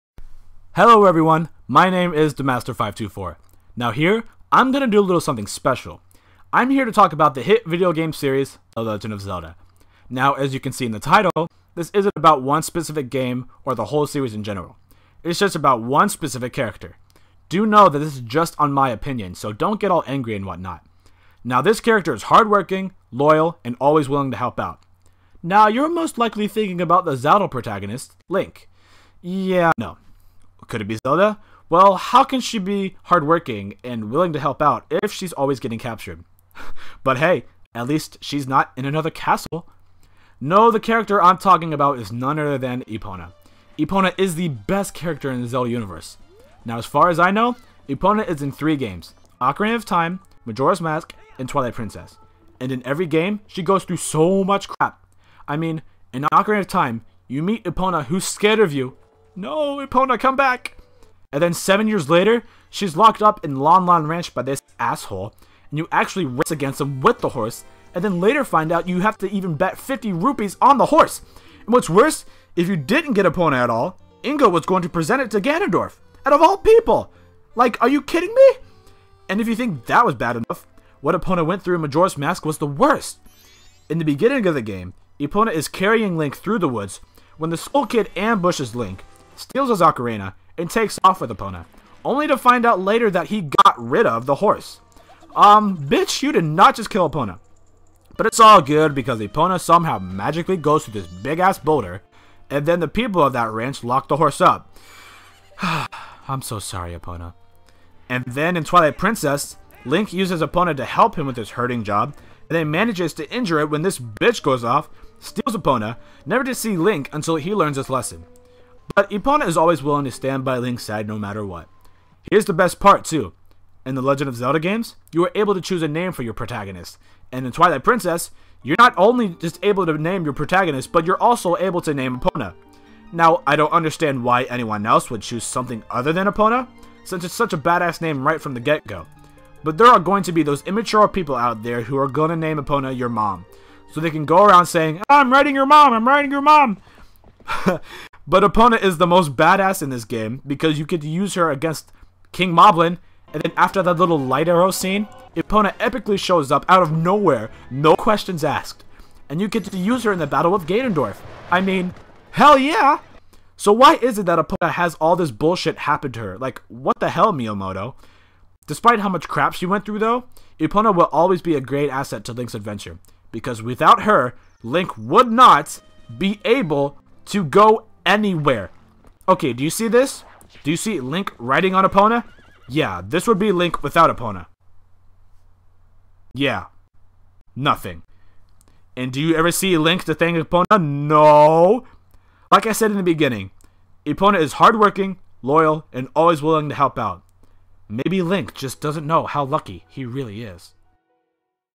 The BEST Zelda character EVER!?
I know it's a bit different than from what I've been doing lately, but I want to try something different. Here, I'm explaining my thoughts. I tried to put some humor in it. I guess. xD Enjoy what you can!